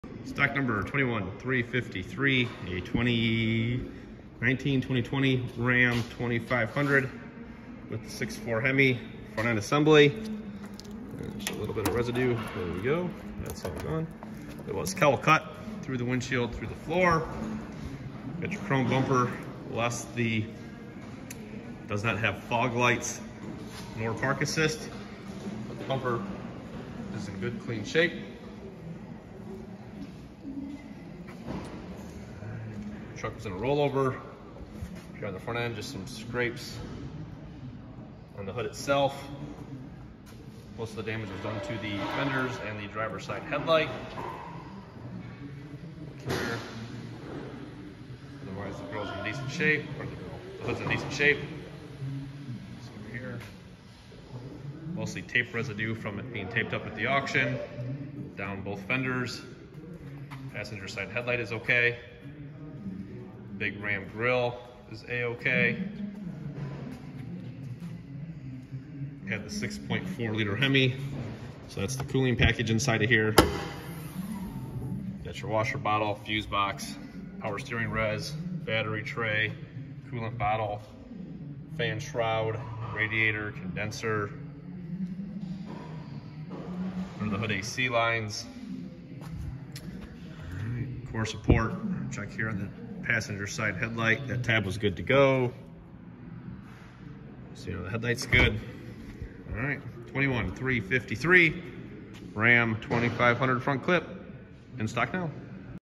2021 Ram 2500 Tradesman 6.4L hemi front clip inspection video August Pohl Auto Parts stock #21353
Parted out a 2021 Ram 2500 Tradesman with a 6.4L hemi engine and this front end assembly.